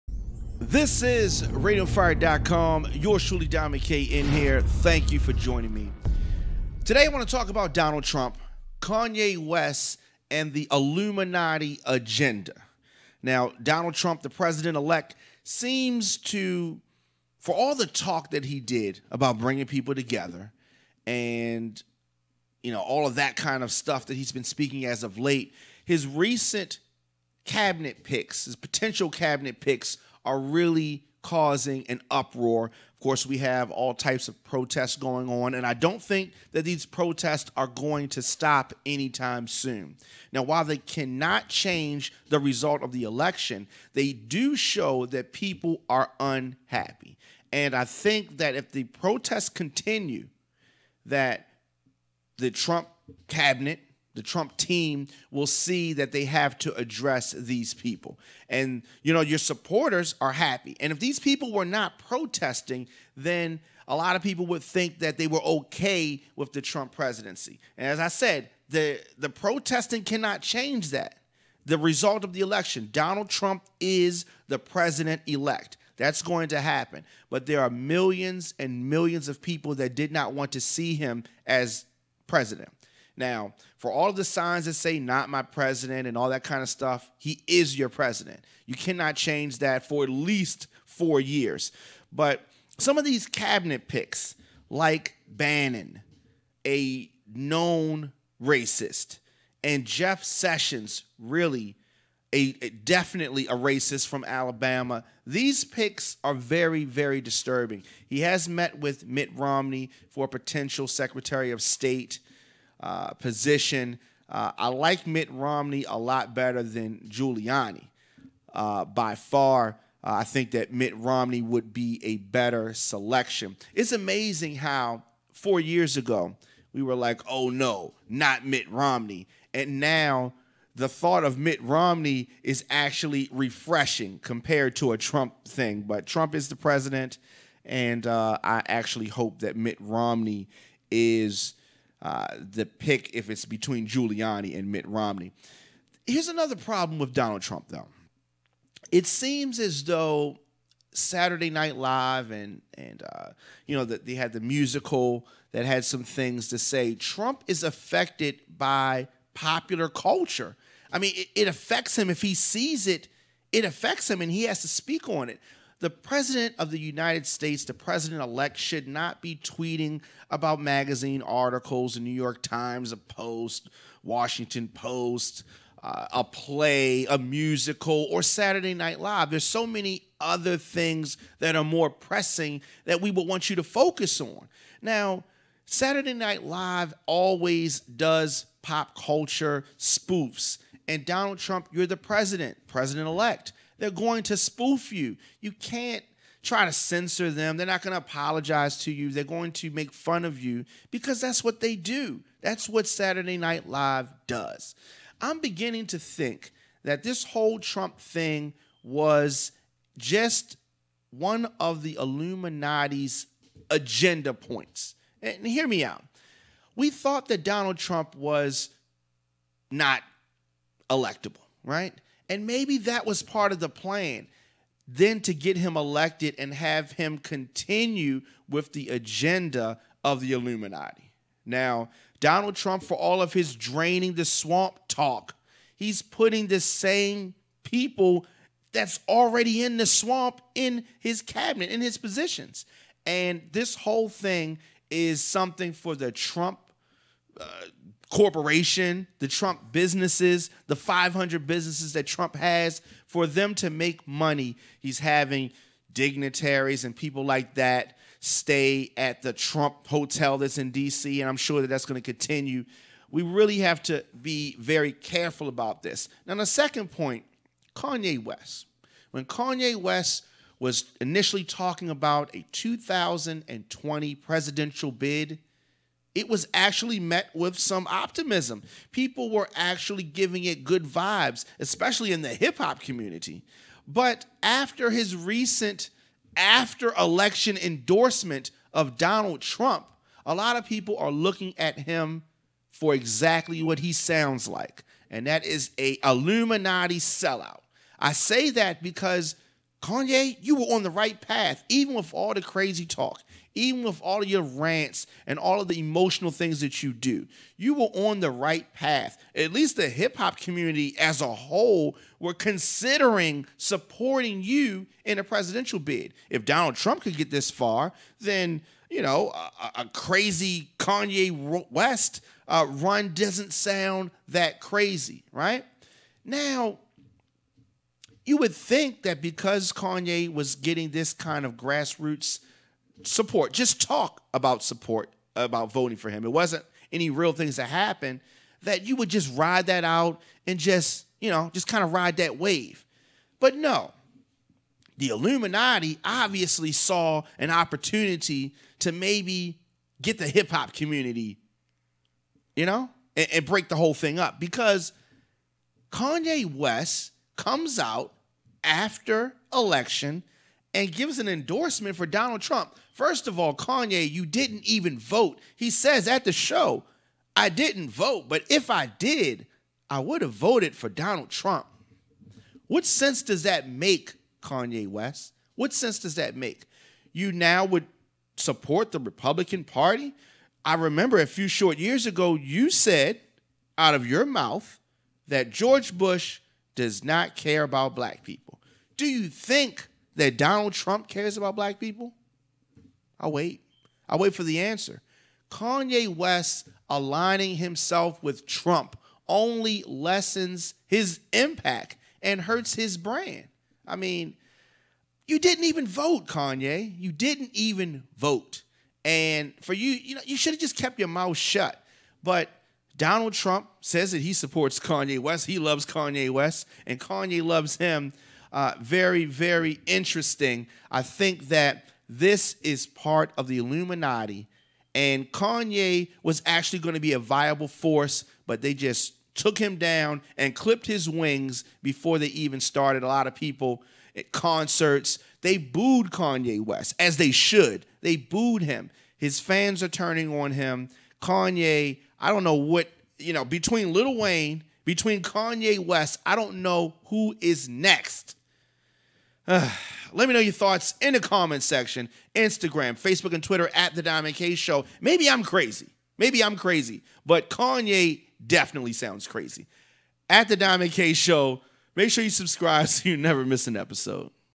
Donald Trump, Kanye West & the Illuminati Agenda
RadioOnFire.com - Diamond K talks about President elect Donald Trump and Kanye West being part of the Illuminati's agenda.